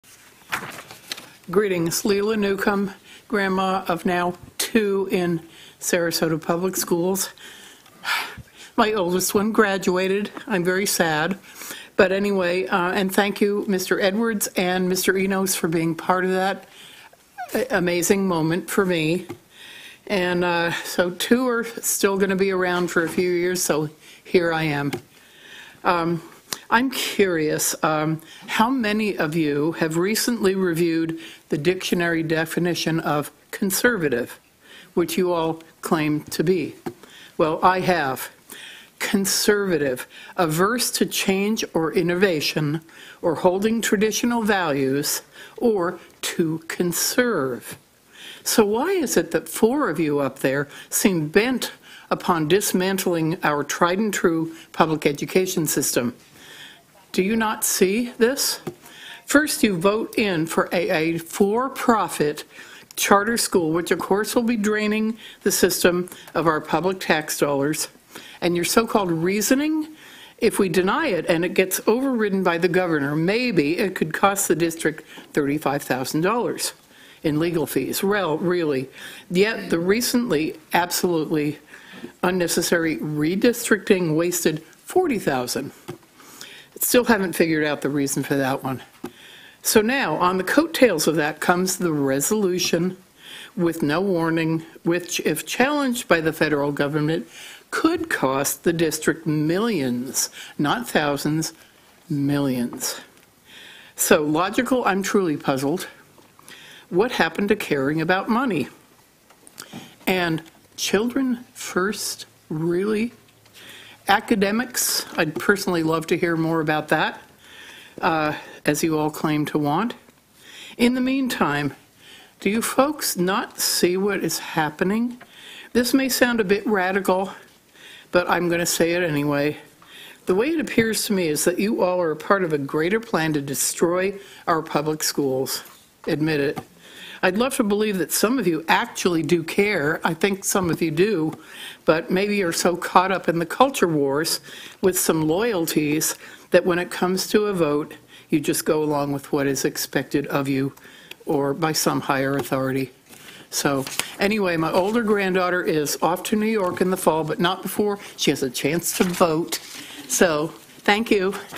Grandma Leila speaks 24 "Conservative" June 4, 2024
Sarasota County School board meeting June 4, 2024
Leila Newcomb speaks on the definition of Conservative